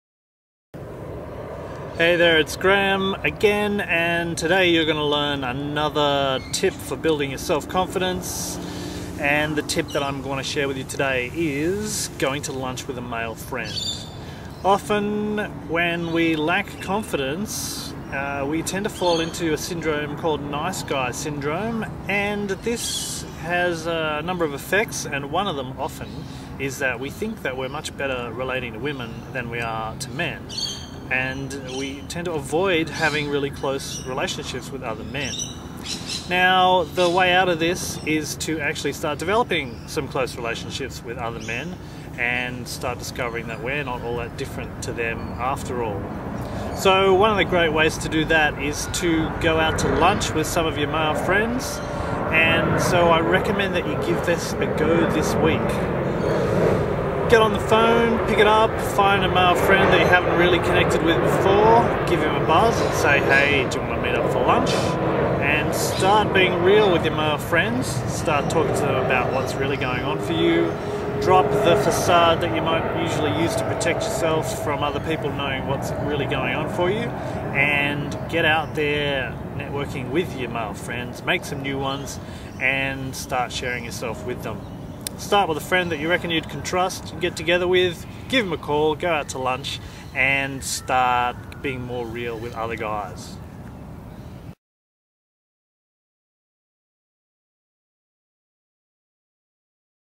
Go To Lunch With A Male Friend
Our relationships with other men can often be relatively shallow. Start to establish deeper relationships by going to lunch with a male friend.

Hey there, it's Graham again, and today you're going to learn another tip for building your self-confidence. And the tip that I'm going to share with you today is going to lunch with a male friend.

Often when we lack confidence, we tend to fall into a syndrome called Nice Guy Syndrome, and this has a number of effects and one of them often is that we think that we're much better relating to women than we are to men and we tend to avoid having really close relationships with other men.

Now, the way out of this is to actually start developing some close relationships with other men and start discovering that we're not all that different to them after all. So one of the great ways to do that is to go out to lunch with some of your male friends, and so I recommend that you give this a go this week.

Get on the phone, pick it up, find a male friend that you haven't really connected with before, give him a buzz, say "Hey, do you want to meet up for lunch?" and start being real with your male friends, start talking to them about what's really going on for you, drop the facade that you might usually use to protect yourself from other people knowing what's really going on for you and get out there, networking with your male friends, make some new ones and start sharing yourself with them.

Start with a friend that you reckon you can trust and get together with. Give them a call, go out to lunch and start being more real with other guys.